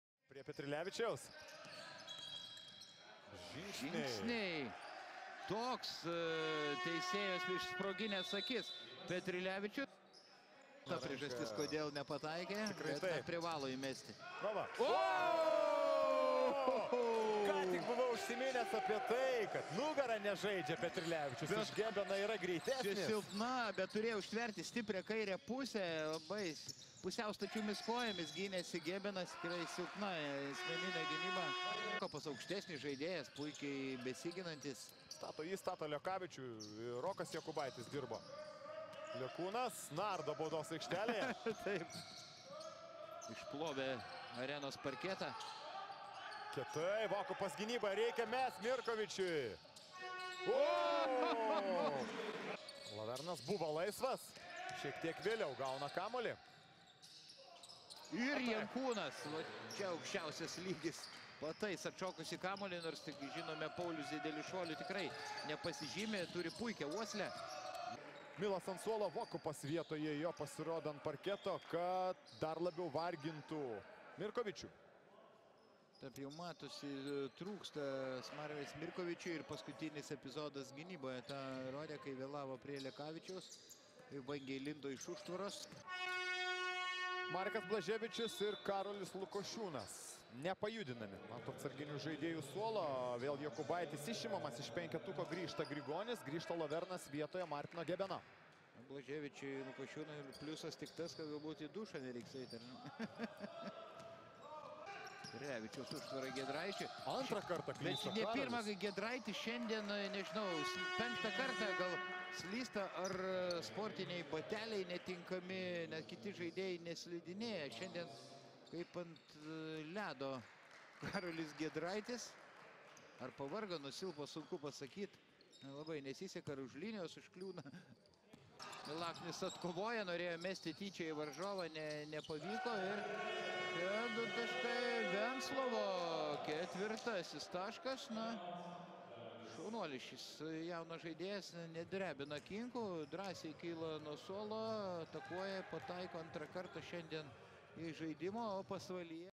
Komentatorių perliukai Pasvalio Pieno Žvaigždės-Kauno Žalgiris
Kmt varžybos, Pasvalio Pieno Žvaigždės-Kauno Žalgiris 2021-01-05
Komentuoja Žilvynas Aleksandravičius ir Vaidas Čeponis.